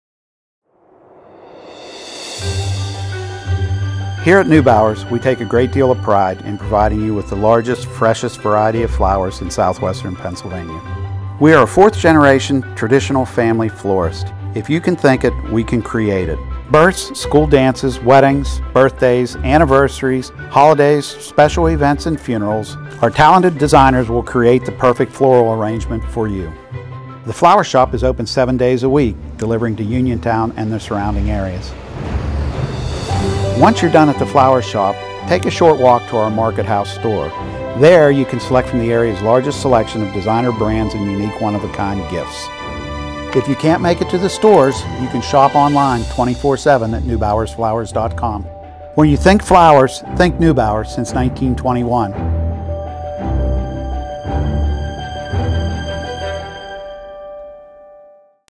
Neubauer's Flowers | Promo
60 second advertisement for their Flowershop and Markethouse in Uniontown, PA. (2018)

Directed/Shot/Edited by
MARCUS MORELLI